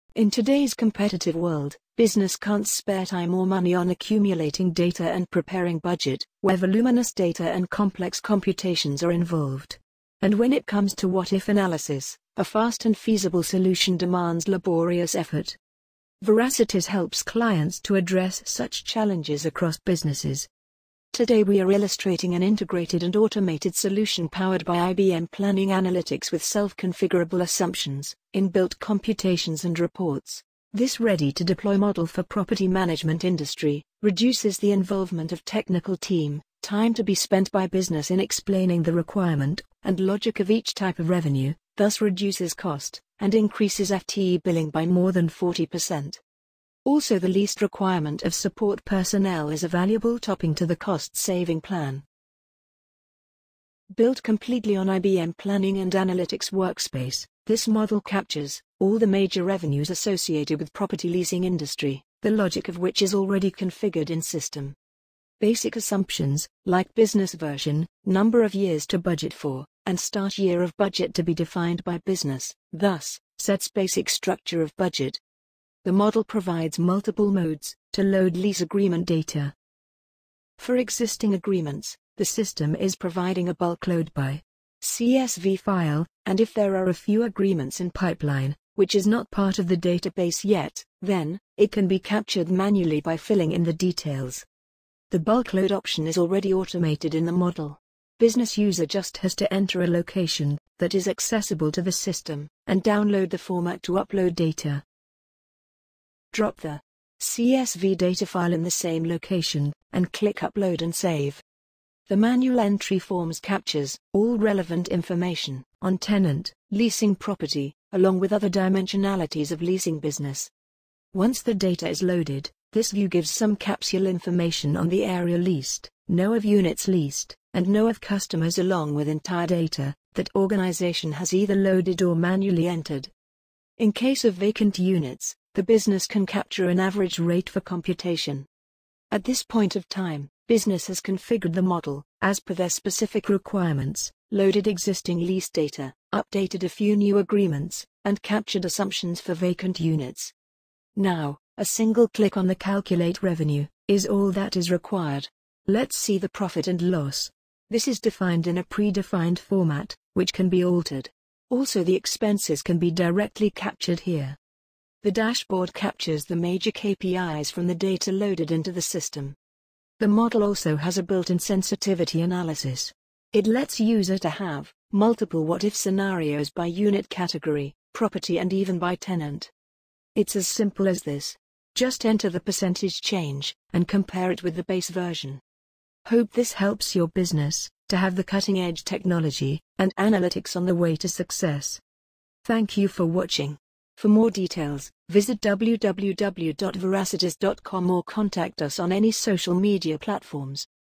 TM1 | PROPERTY LEASING | LEASE RENTAL | IBM PLANNING ANALYTICS | MALL MANAGEMENT
The rental and leasing business growing so fast around the world with an average 22% growth year on year. The Ownership model is getting changed to Leasing. There is a need of efficient tracking and managing the every unit lease profiles, rental charges, consolidated statements for each leased property, Analysis of occupancy etc. IBM Planning analytics would change the lease management done in today's time with a spreadsheet way. Improving the process of planning, scenario building, lease tracking, and reporting and consolidation of data in real-time. The model build by Veracitiz as Property Leasing can be used by Malls, Property Rentals, Land Leasing and heavy equipment leasing companies too.